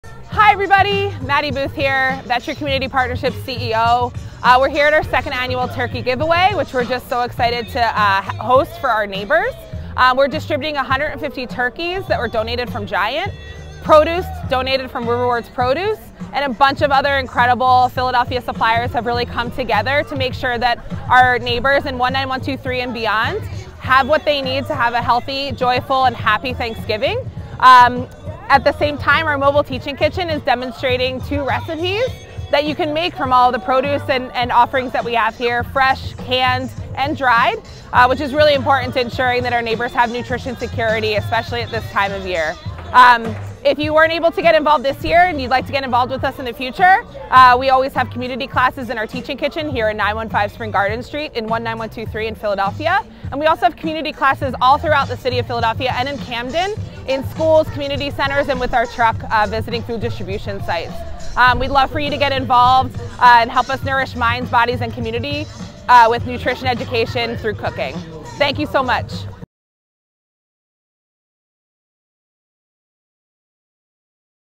2023 Turkey Giveaway
We are so honored that we get to be a part of nourishing the minds and bodies of our community. Wishing our 19123 neighbors and beyond a very happy holidays!